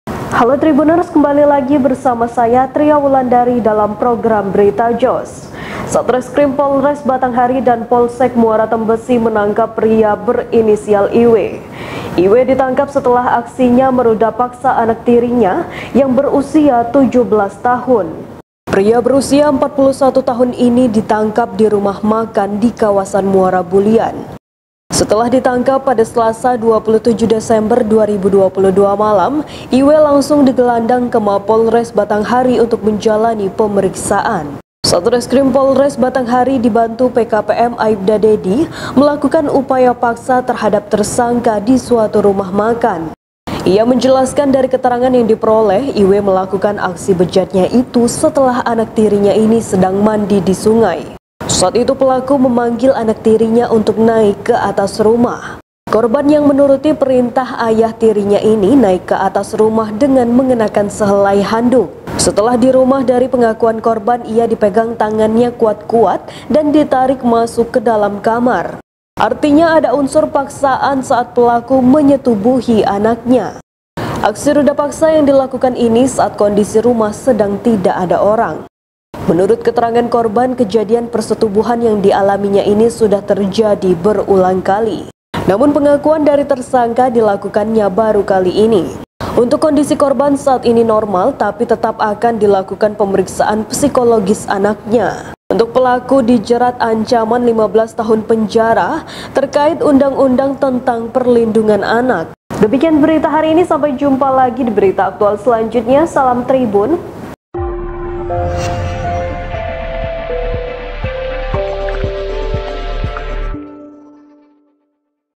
Tak Tahan Lihat Anaknya Pakai Handuk, Ayah di Batanghari Nekat Rudapaksa Anak Tirinya
Satreskrim Polres Batanghari dan Polsek Muara Tembesi menangkap pria berinisial IW.

IW ditangkap setelah aksinya merudapaksa anak tirinya yang berusia 17 tahun. (ria)